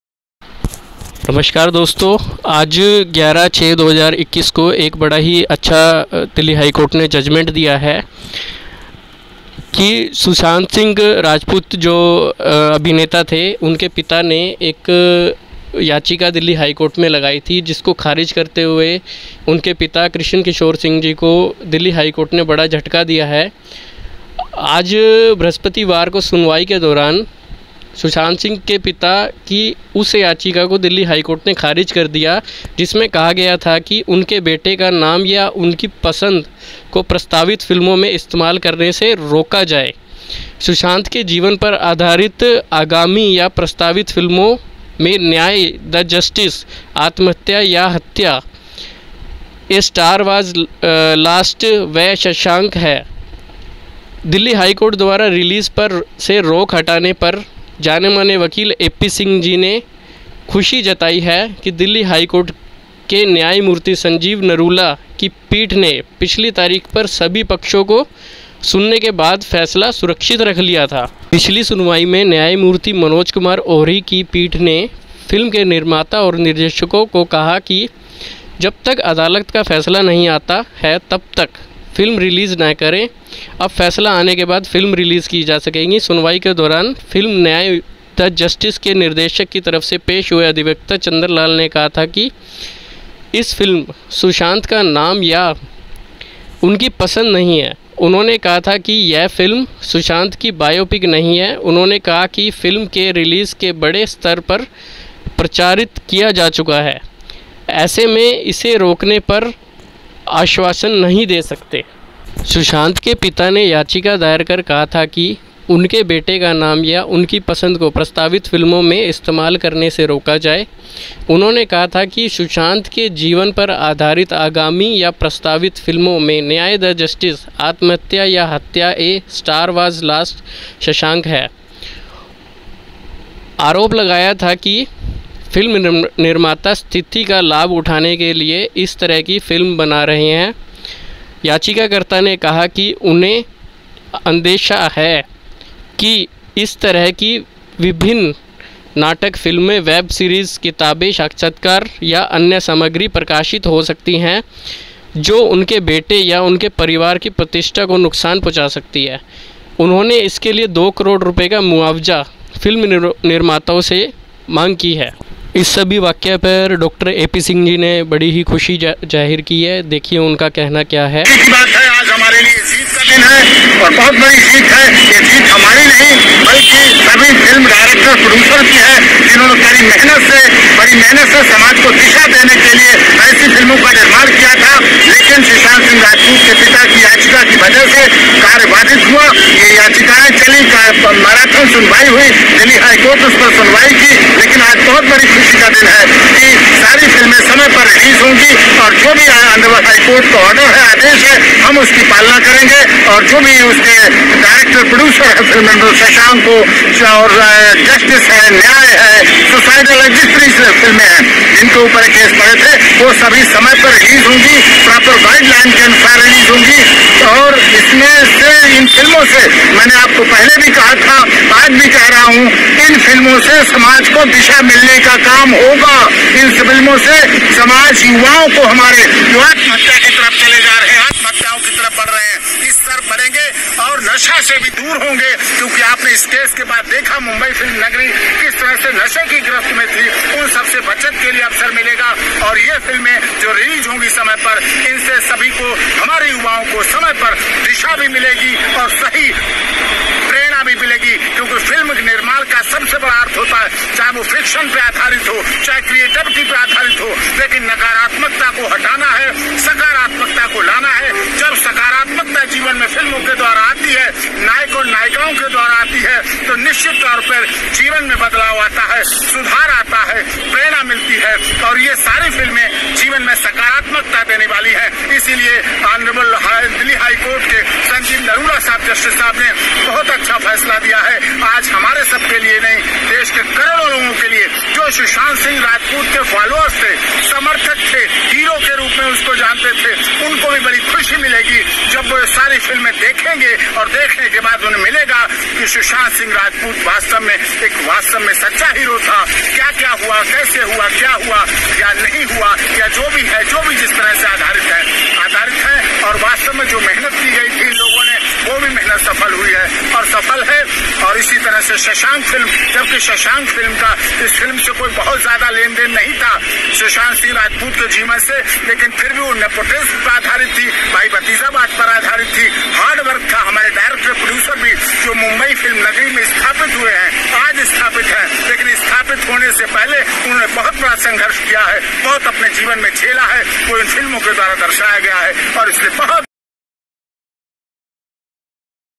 सुशांत सिंह राजपूत के पिता को झटका,फिल्मों पर रोक से दिल्ली HC का इनकार,Dr A.P Singh जी ने जताई खुशी
लड़कियों को ना दे मोबाइल फोन बढ़ते अपराध का सबसे बड़ा कारण।
 U.P महिला आयोग।
बयान का असली मतलब सुनिए।

Dr A.P Singh जी ने जताई खुशी।